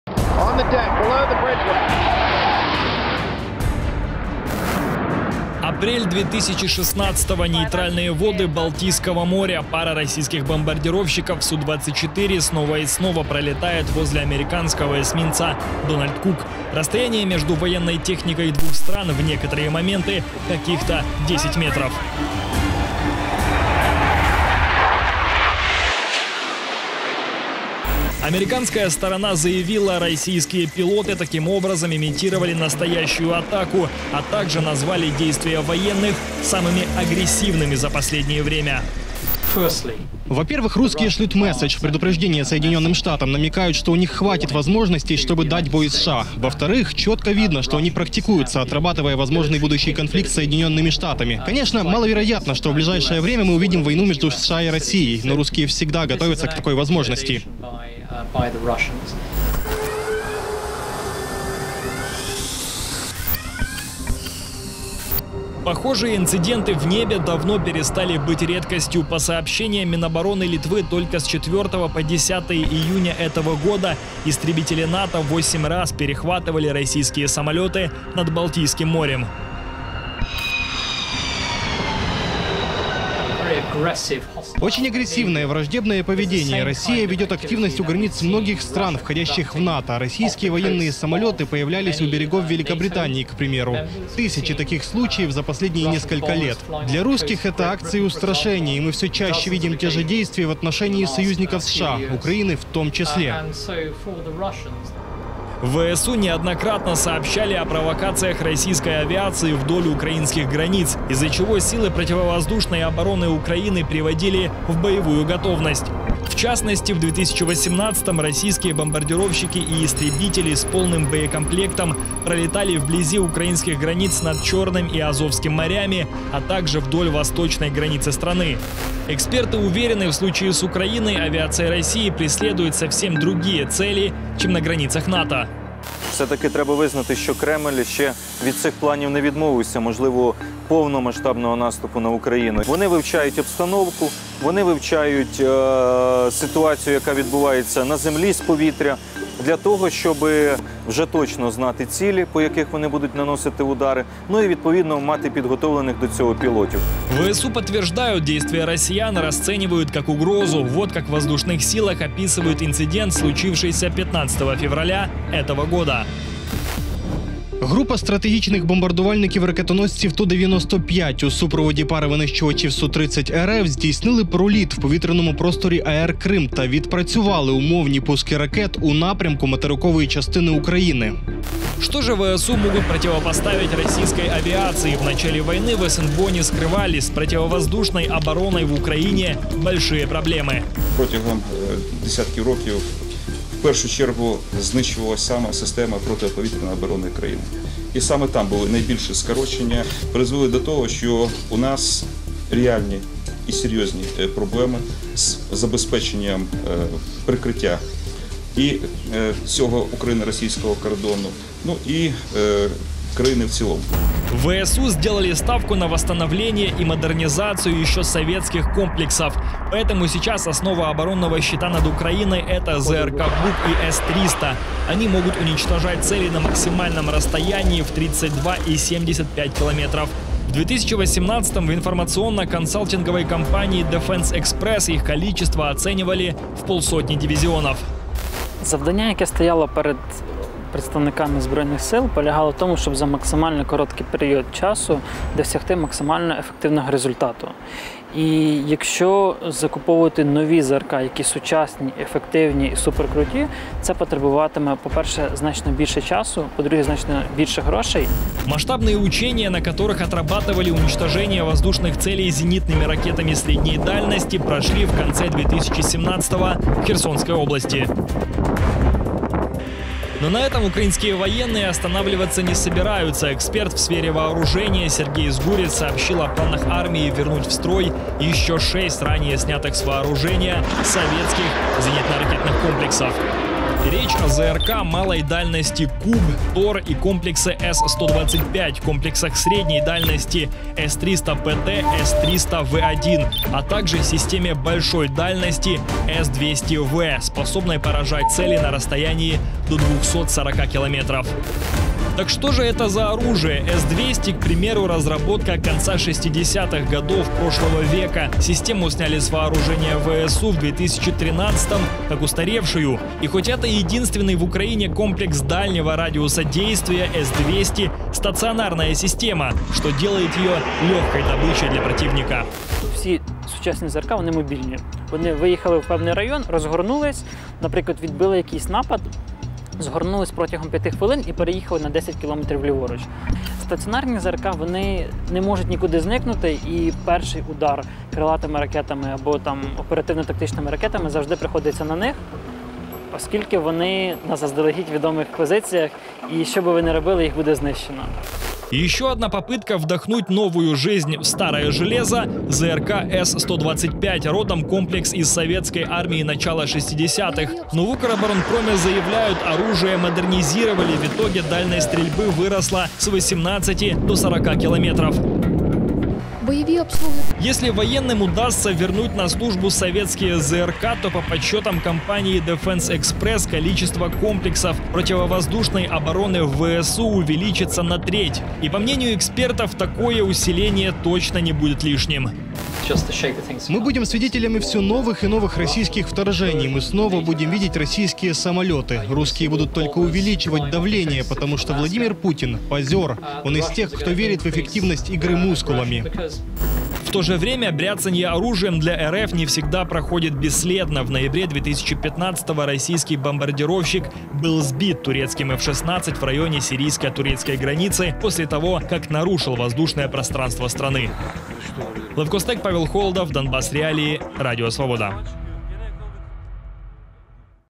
Военные самолеты России у границ Украины | Донбасc.Реалии
Российская авиация и угроза с воздуха. Самолеты РФ все чаще замечают у границ стран НАТО. Последний известный случай - истребители США перехватили российские бомбардировщики недалеко от Аляски. В то же время, местом повышенного внимания российских военных самолетов остается Украина. Что сейчас происходит в небе над украинскими границами и каким может быть ответ на воздушную угрозу?